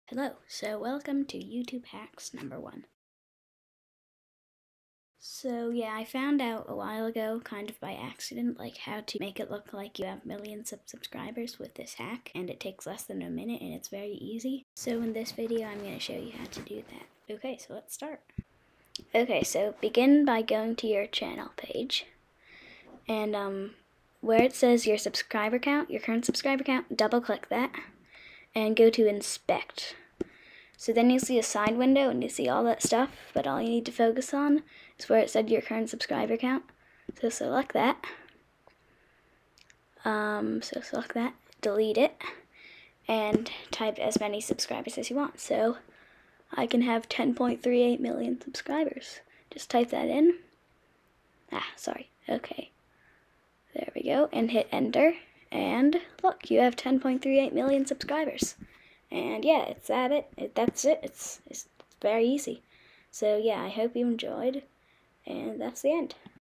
How to get millions of subscribers in less than 60 seconds! (YouTube Hack 1)
In this video I show you how to get millions of subscribers with one easy hack! No, this is not clickbait.  Watch the video for proof!

This is the first of many episodes of my easy YouTube hacks series.